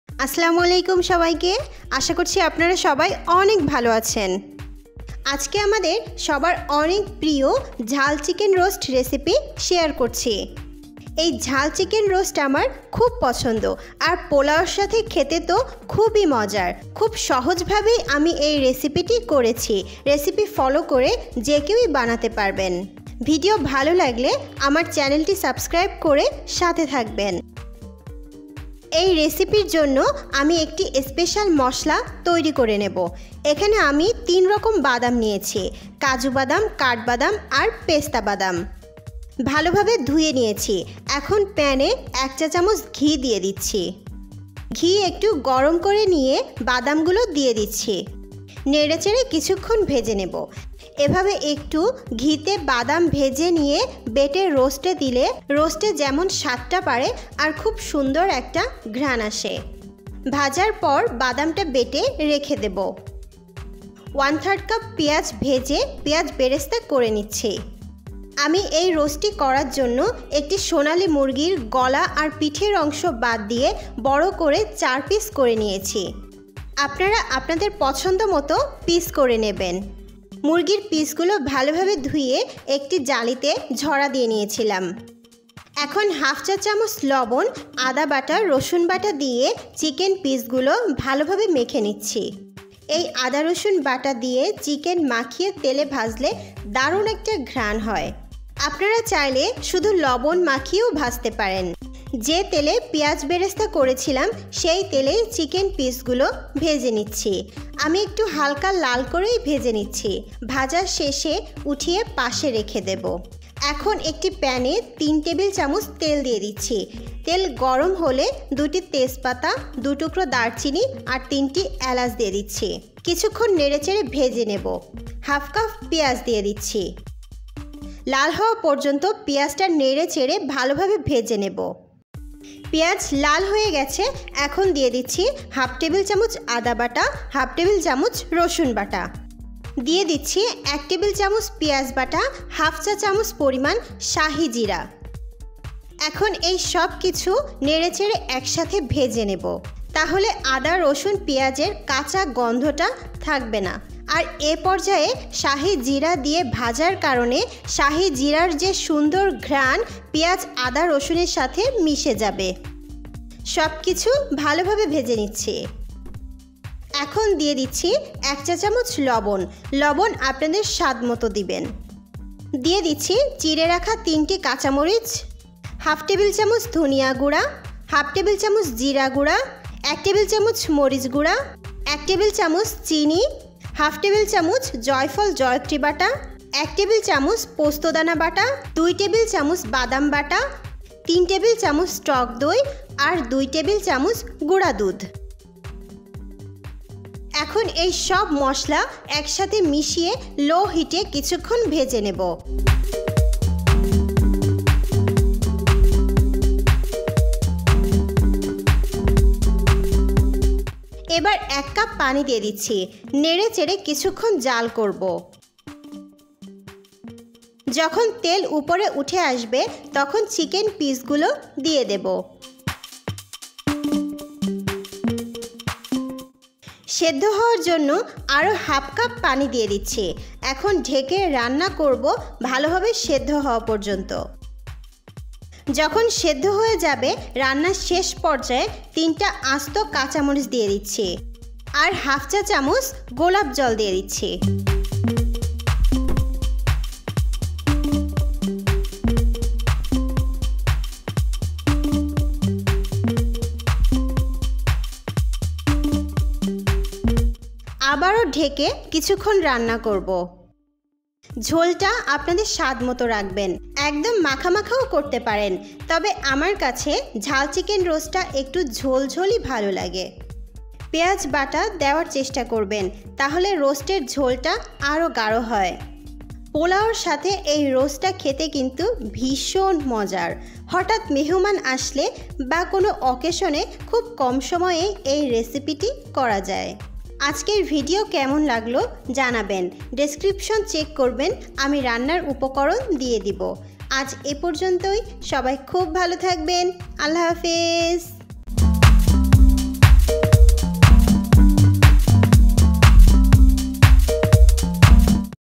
সবচেয়ে সহজে শাহী চিকেন রোস্ট রেসিপি/Easy Chicken Roast/Traditional Chicken Roast /ঝাল চিকেন রোস্ট
☑️রান্নার উপকরণঃ

🔷মুরগি (Chicken)- 4 pcs
🔷আদা বাটা (Ginger paste) -১/২ tbs
🔷রসুন বাটা (Garlic paste )--১/২ tbs
🔷পেঁয়াজ বাটা (Onion paste)- 1tbs
🔷শাহী জিরা (Royal Cumin) -১/২ tsp
🔷মরিচ গুড়া (Red Chilli powder) -1tbs
🔷ধনিয়া গুড়া (Coriander powder)-1/2 tbs
🔷জিরা গুড়া (Cumin Powder)-১/২tbs
🔷পোস্ত দানা বাটা (Poppy-seed Paste)-1tbs
🔷জায়ফল জয়ত্রী বাটা (nutmeg Mace paste)-1/2tbs
🔷দারচিনি (Cinnamon Stick) - 2 pcs , তেজপাতা (bay leaf) - 2 pcs
🔷সবুজ এলাচ (Green Cardamon) - 3 pcs
🔷কাঁচামরিচ (Green Chilli ) - 5-6 pcs
🔷চিনি (Sugar) - 1 Tbs
🔷বাদাম বাটা nut paste) - 2 Tbs
🔷টক দই (sour yogurt) - 3 Tbs
🔷ঘি (Ghee) - 1 tsp
🔷লবণ  - to taste
🔷গোলাপ জল (Rose Water)
🔷তেল (Oil) - 1/2 Cup  (total)
🔷পেঁয়াজ বেরেস্তা (Fried Onion) - 1/4 Cup
🔷গুড়া দুধ (Powder Milk)- 2tbs